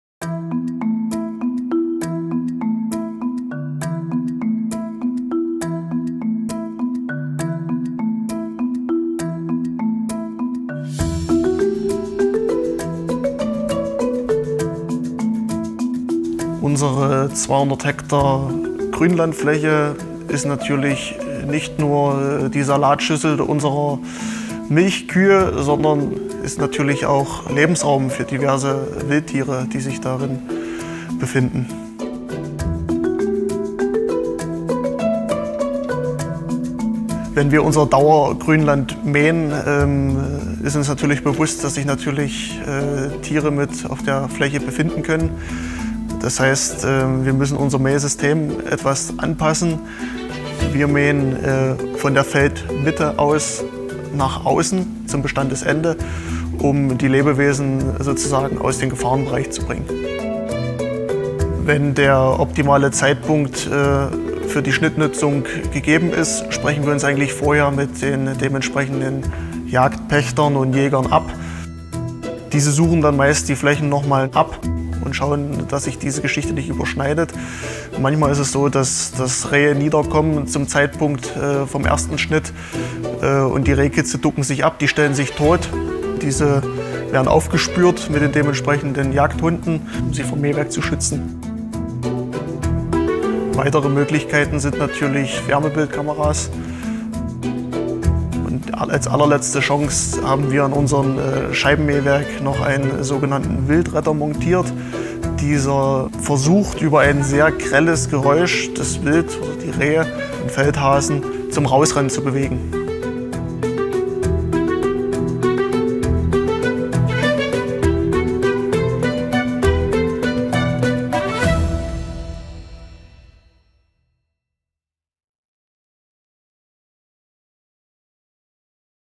Schutz von Wildtieren: Landwirte auf den Spuren der Rehkitze
Die Grünlandflächen des Lehr- und Versuchsgutes Köllitsch sind die „Salatschüssel“ der dort gehaltenen Nutztiere und werden mehrmals im Jahr gemäht. Gleichzeitig ist diese Fläche der Lebensraum verschiedener Wildtiere. Mit tierschonendem Mähen, angepassten Verfahren und optimierter Technik vermeiden die Mitarbeiter Schäden und Verluste an Wildtieren.